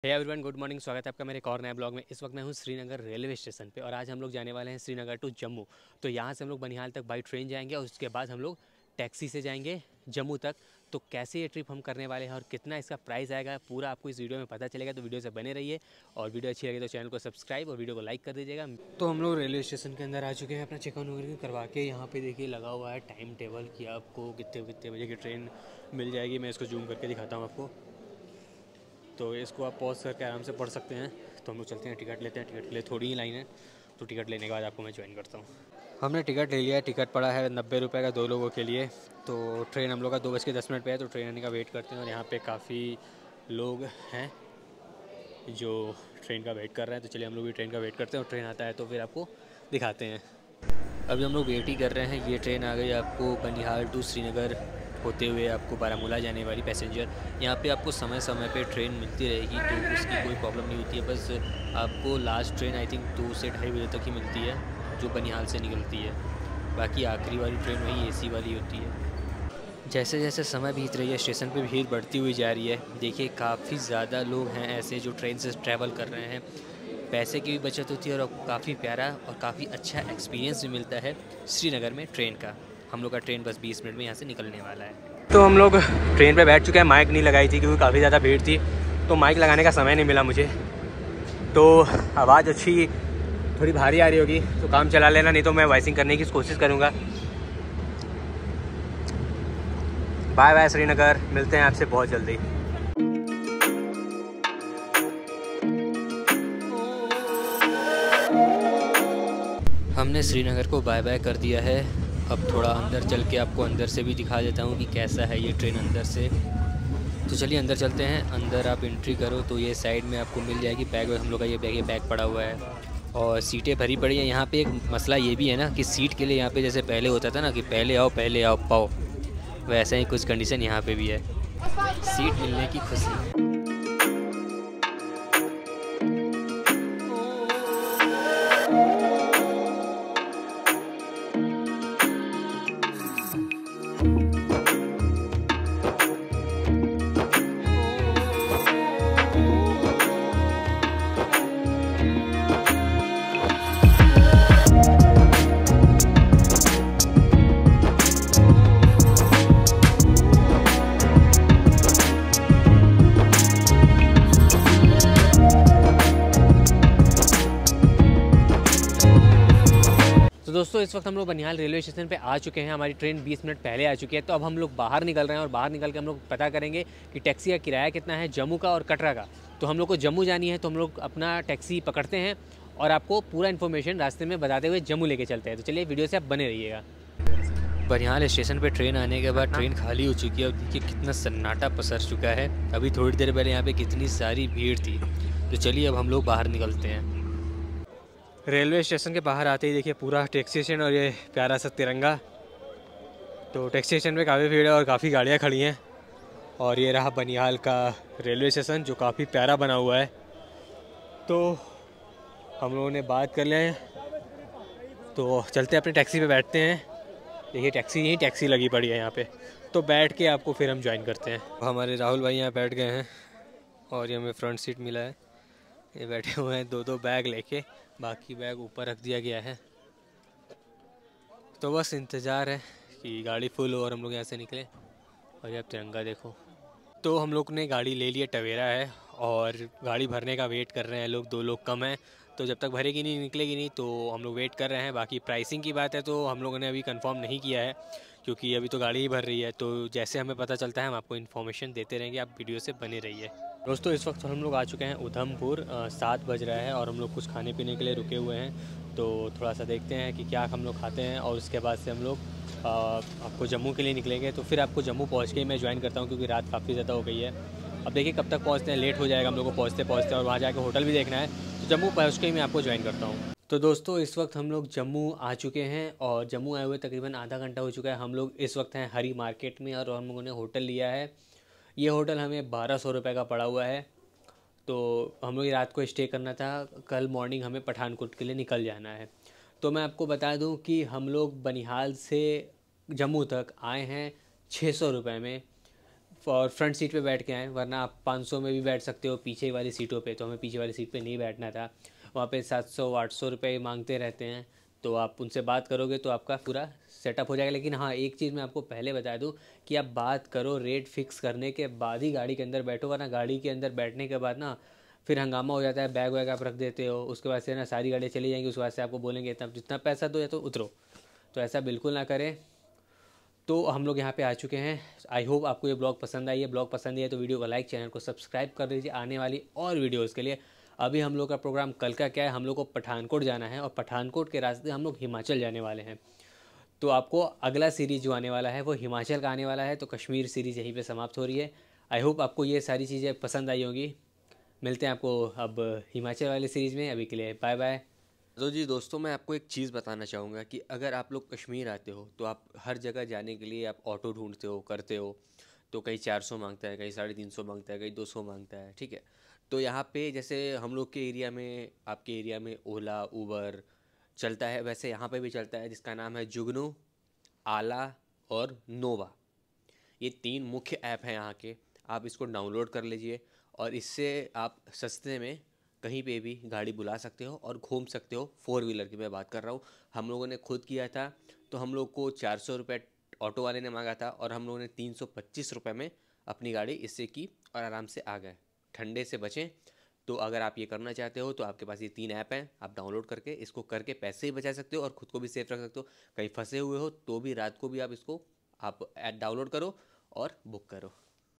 SRINAGAR TO BANIHAL BY TRAIN | SRINAGAR TO JAMMU | FULL INFORMATION
Doston aap is video me Shrinagar se Jammu tak ki yatra dekhenge ki kaise hamne ye yatra kiya aur aap kam budget me kaise Shrinagar se Jammu tak ki yatra kar sakte hain